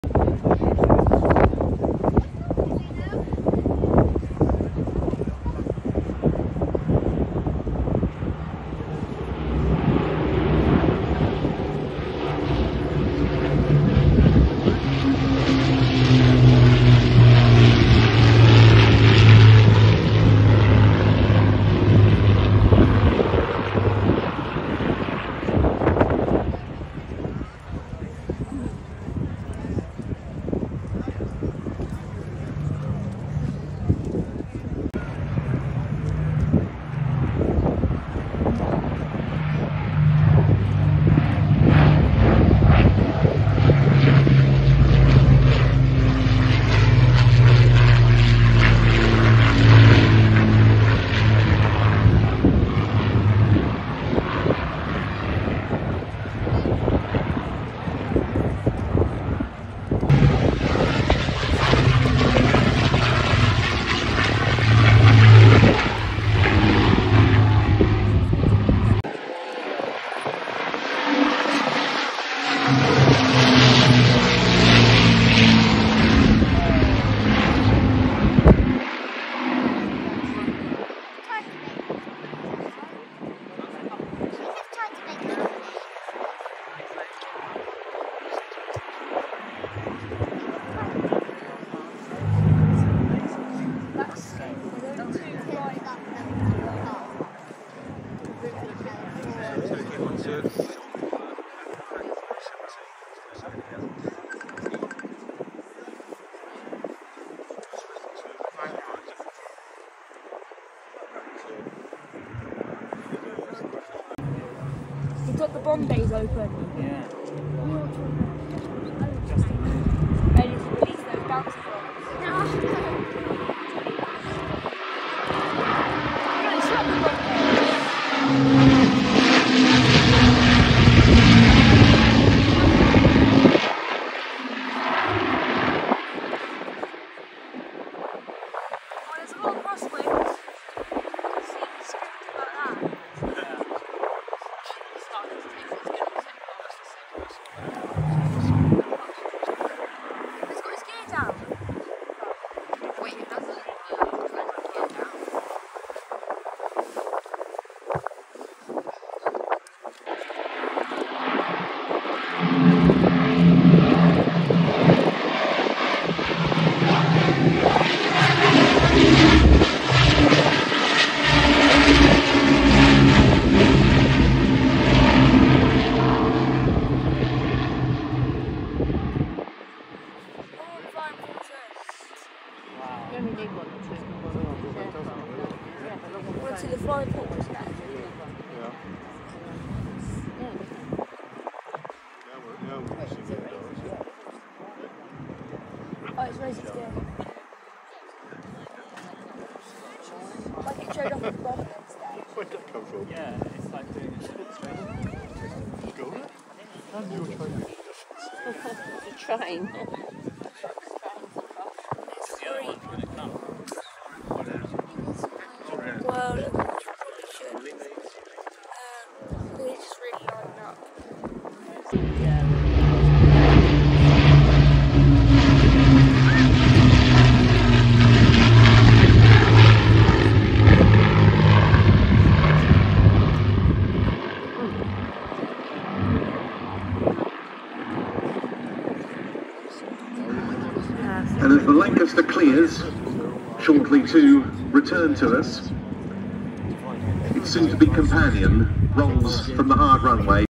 AVRO Lancaster
The Avro Lancaster, commonly known as the Lancaster Bomber, is a British Second World War heavy bomber. It was designed and manufactured by Avro as a contemporary of the Handley Page Halifax, both bombers having been developed to the same specification, as well as the Short Stirling, all three aircraft being four-engined heavy bombers adopted by the Royal Air Force (RAF) during the same era.